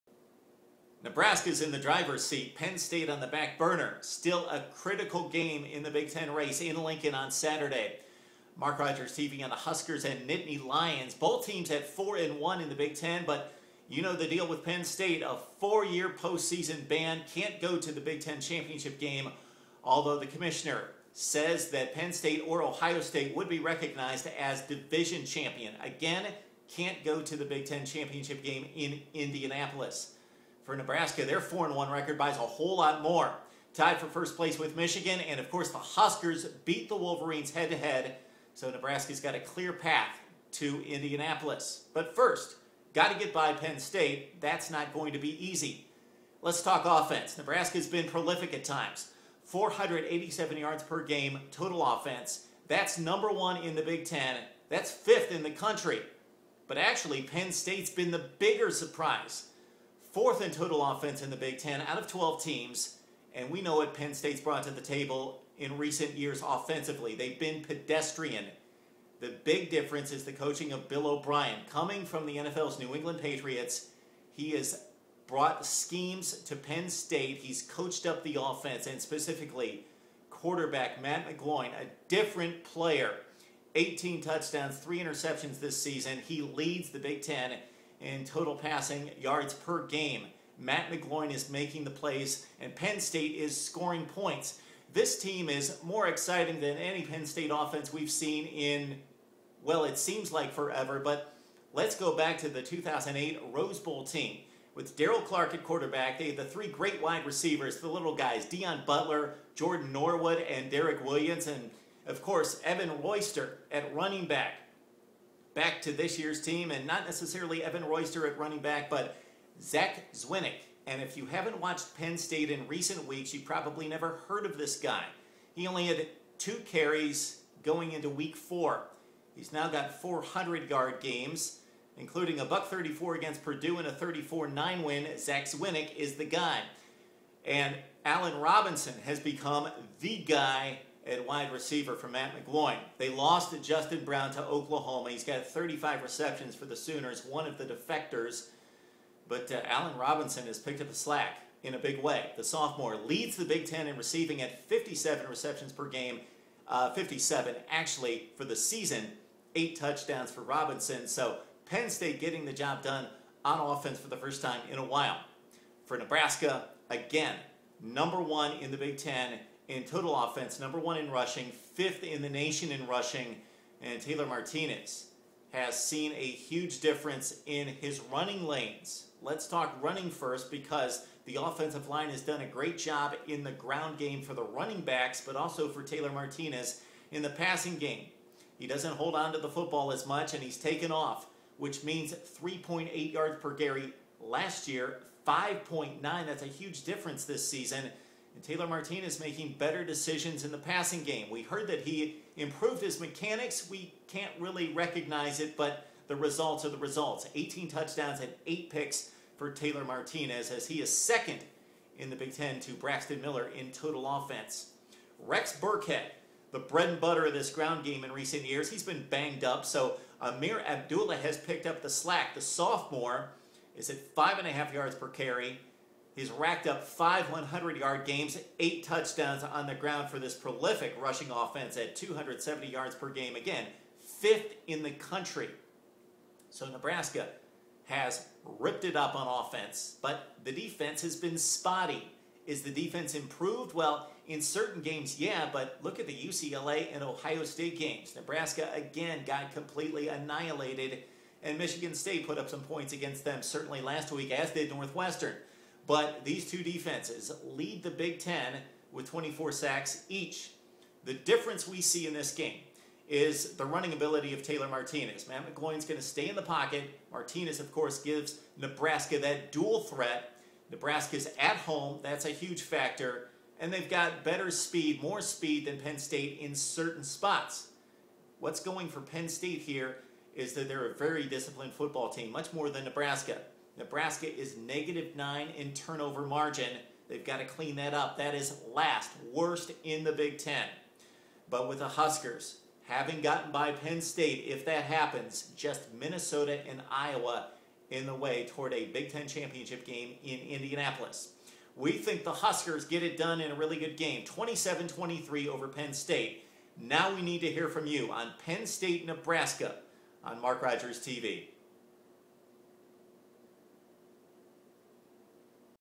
Nebraska Cornhuskers - Penn State Nittany Lions Preview & Prediction / Taylor Martinez, Matt McGloin
MarkRogersTV breaks down a crucial game in the Big Ten race with Penn State at Nebraska